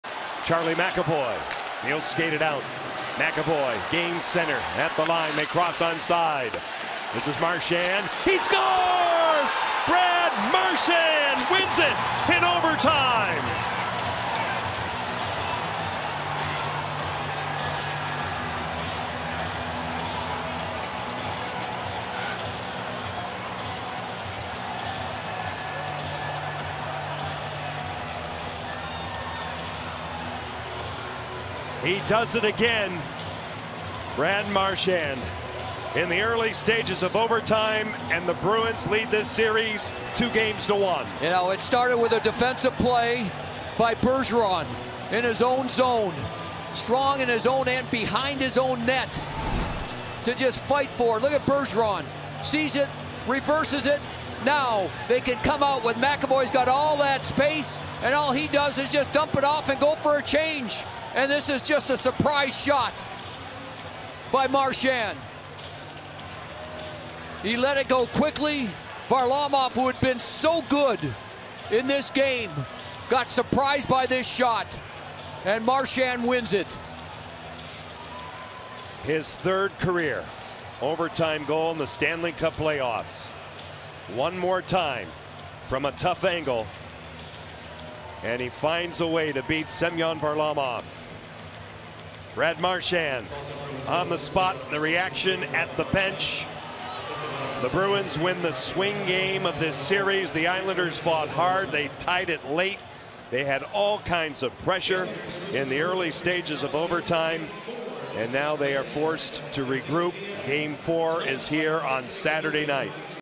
Marchand roofs OT Winner past Varlamov in Game 3
Boston Bruins forward Brad Marchand roofs a shot into the top corner of the net past New York Islanders goalie Semyon Varlamov to win Game 3 in overtime.